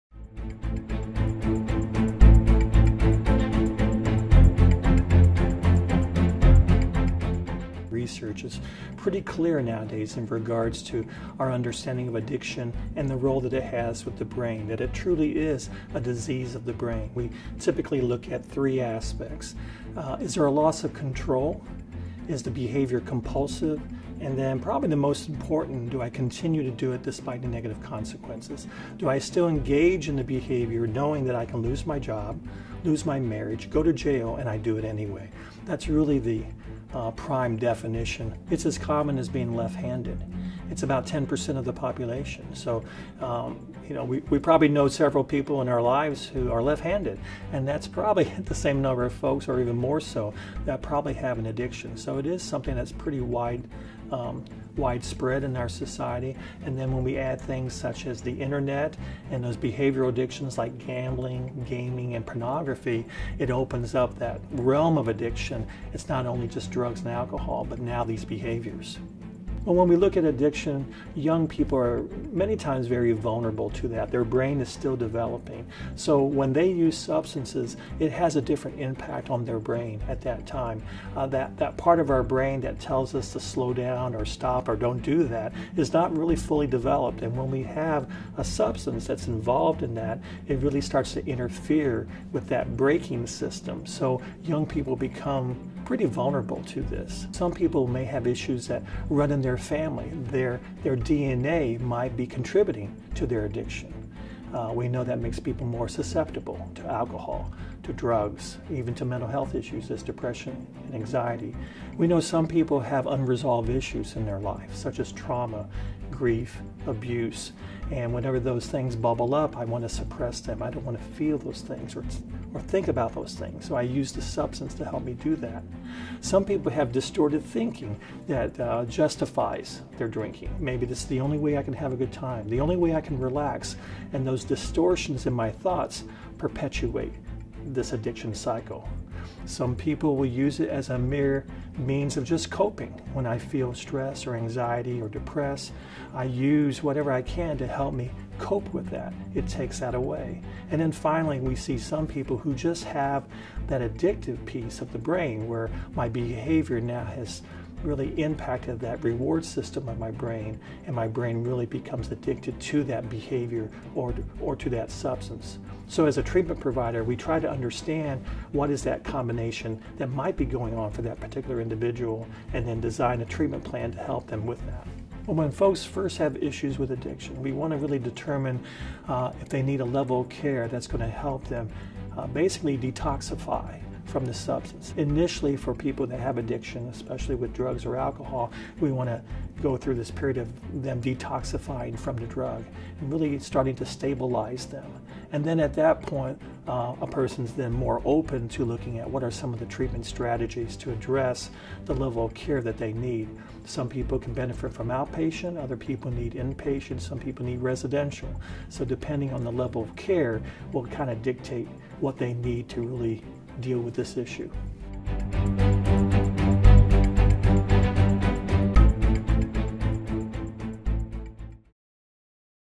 Start the Conversation: Insights on Addiction
Hear from Dr. Chris Tuell, Clinical Director of Addiction Services at the Lindner Center of Hope, as he shares insights on addiction.

Start the Conversation was developed as a community partnership with Lindner Center of HOPE, NAMI, and TiER1 Performance. For additional educational materials related to mental health visit: